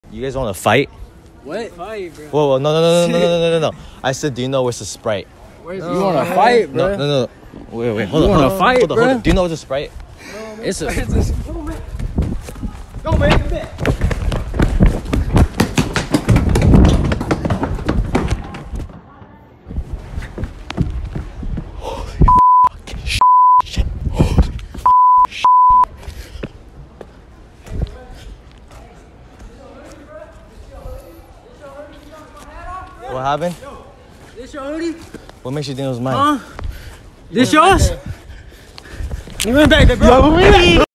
Asking Drunk Strangers If They Want To Fight And Changing Sweater 😂
Asking drunk people if they want to fight and then changing sweater on them so they don’t know who I am 😂